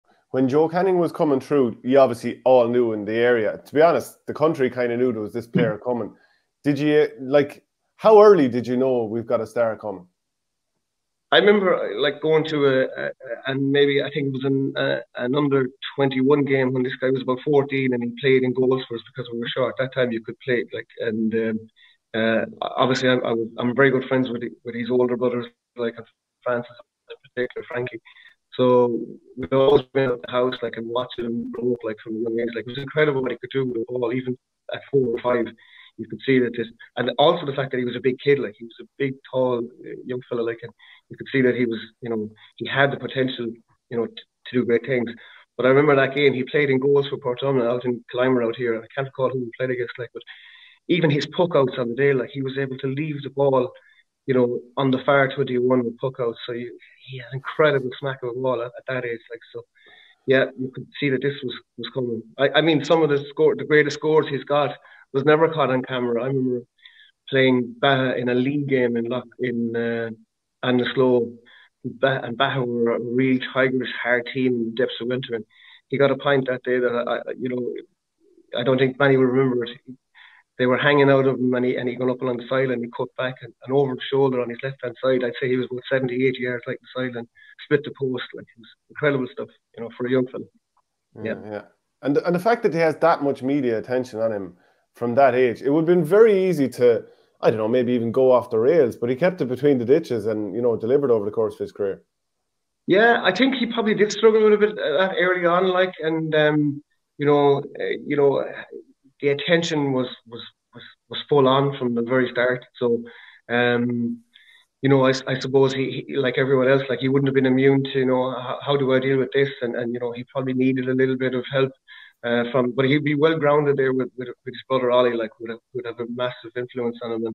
"Joe Canning aged 4 or 5... incredible what he could do with a ball"
Portumna native Johnny Kelly recalls Joe Canning as a child prodigy.
See the full show over on the OurGame Supporters Club for €5 per month